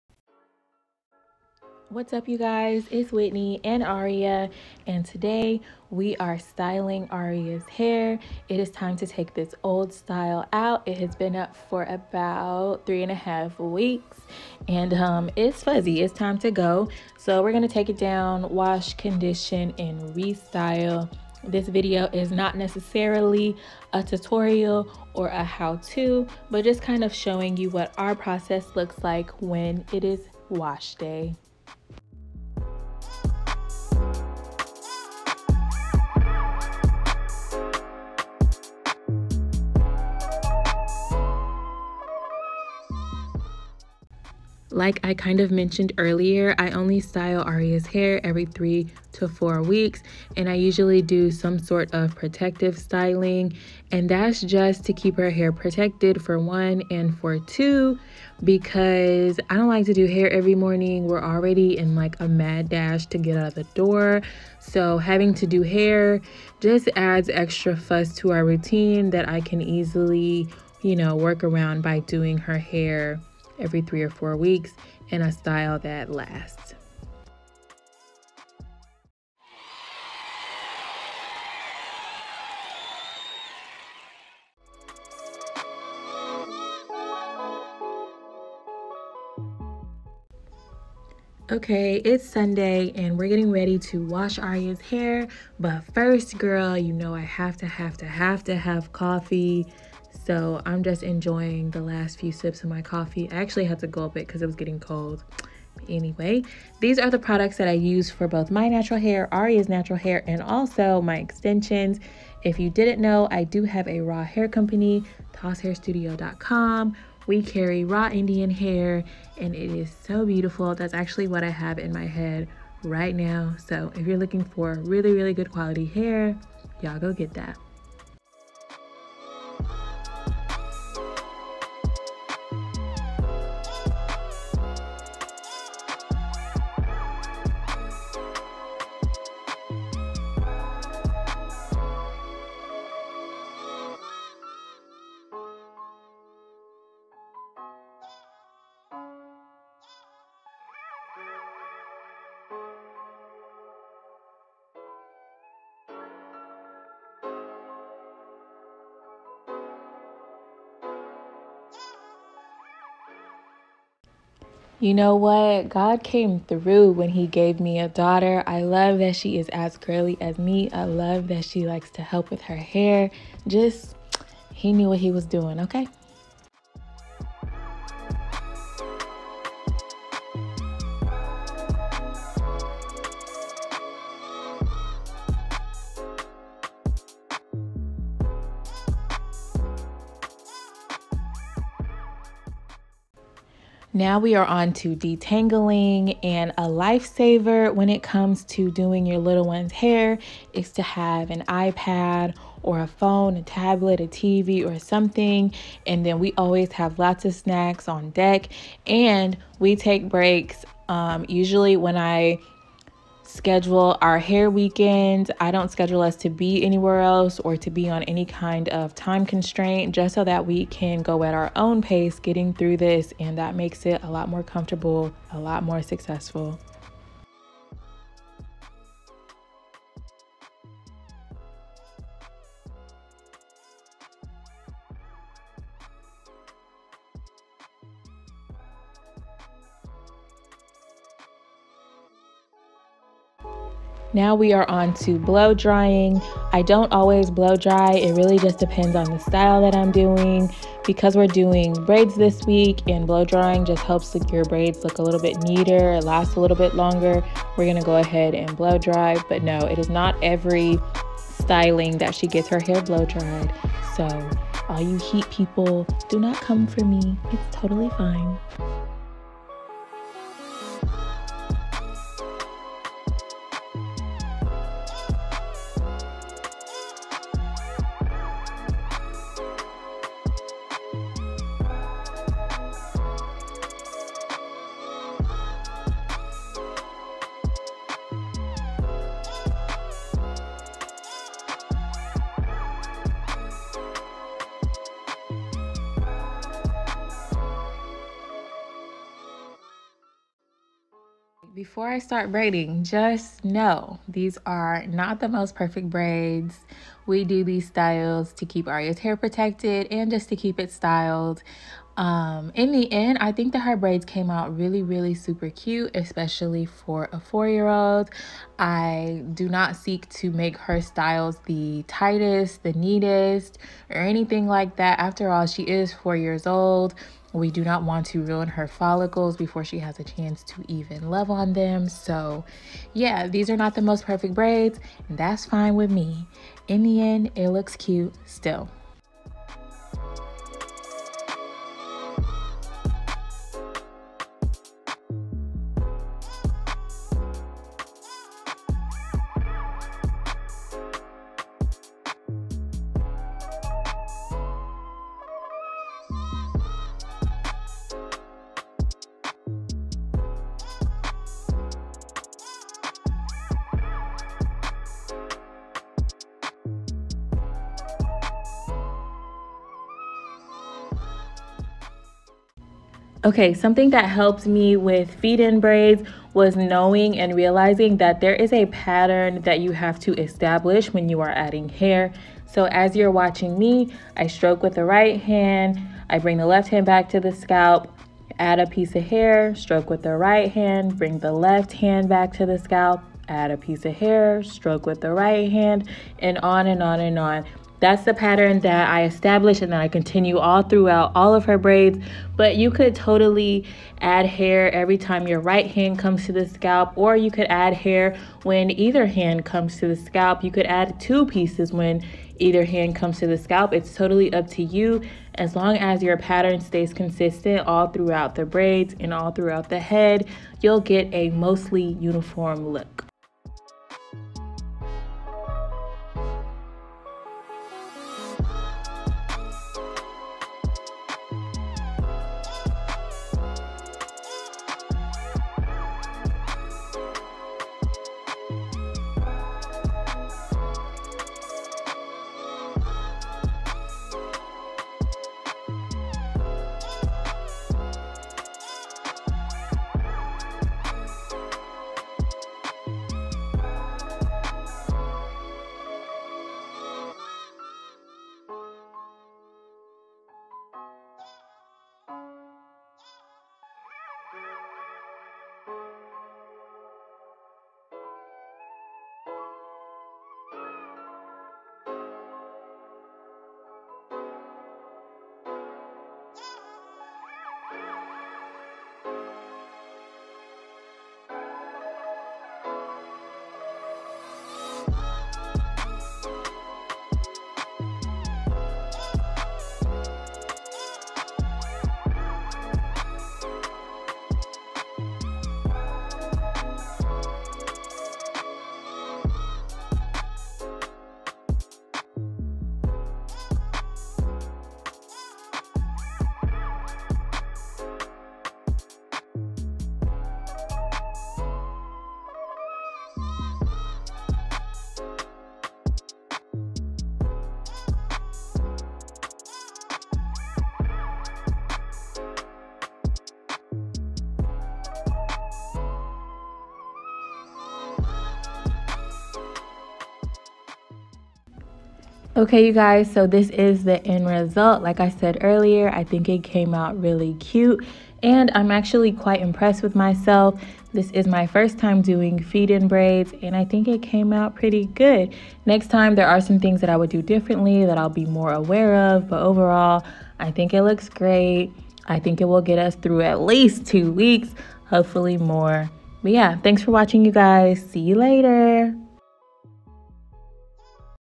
Little Girl's Natural Hair Wash Day | Lemonade Braids
Hey ya'll! Today I'm restyling my 4 year old's hair into a protective style that I've been dying to try... LEMONADE BRAIDS.

As some of you can relate styling a little girl's natural hair can be a TASK but don't fret I drop some gems in this video while showing you our wash day process.